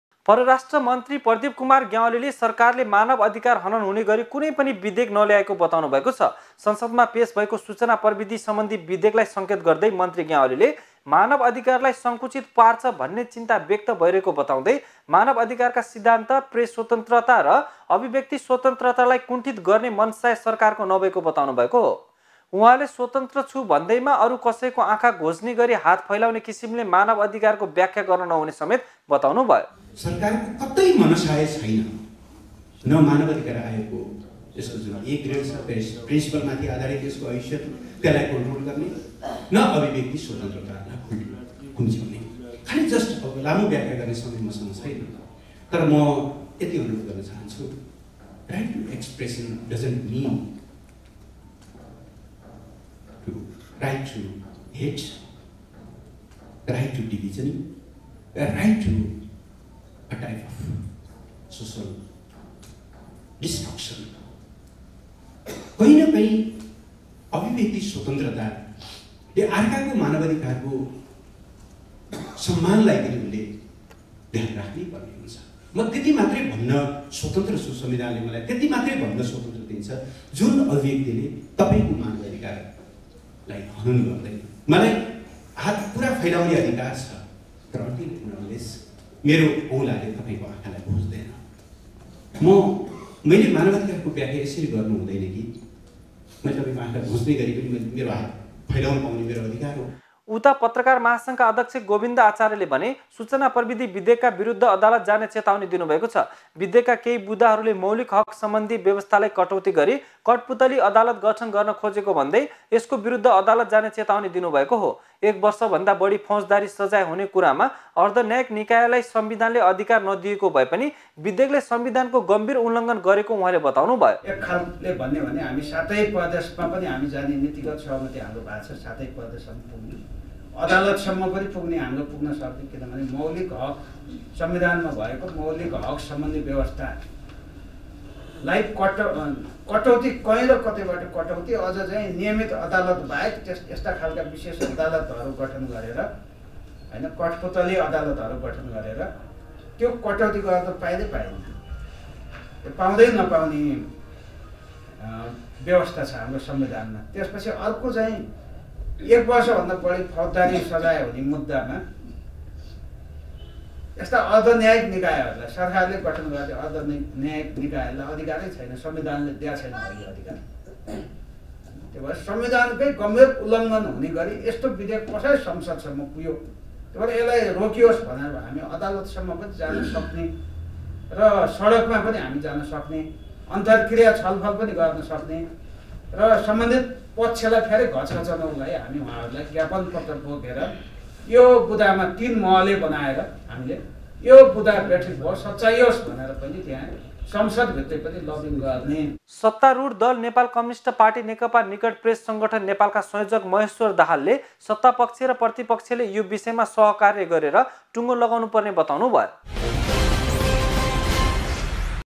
सूचना प्रविधि विधेयकका विरुद्ध अदालत जाने पत्रकार महासंघको चेतावनी || Sungava Khabar
मानव अधिकार हनन हुनेगरि कुनैपनि विधेयक नल्याएको सरकारको दावी, सूचना प्रविधि विधेयकका विरुद्ध अदालत जाने पत्रकार महासंघको चेतावनी ।।